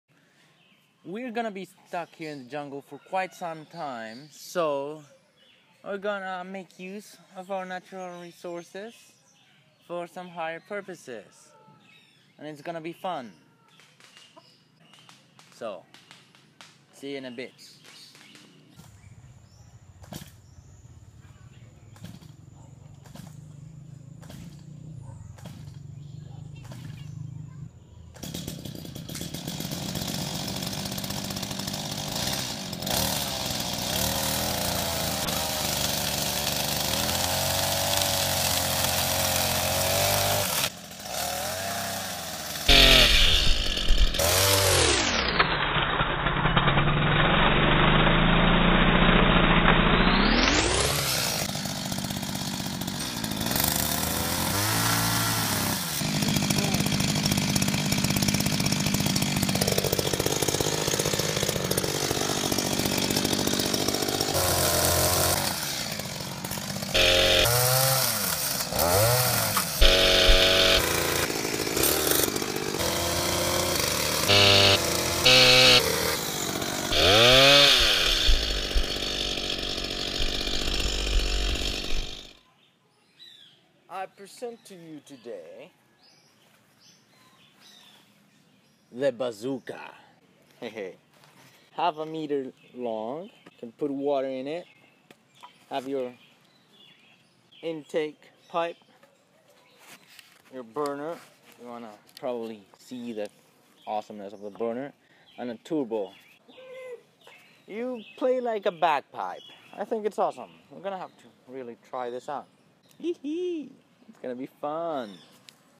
Bazooka Bamboo Bong
what do you do when you're stuck in the jungle with nothing but a chainsaw and lots and lots of bamboo? Well, you build a bamboo bong, and not just any bong, but a ginormous, gargantuan bazooka bong :)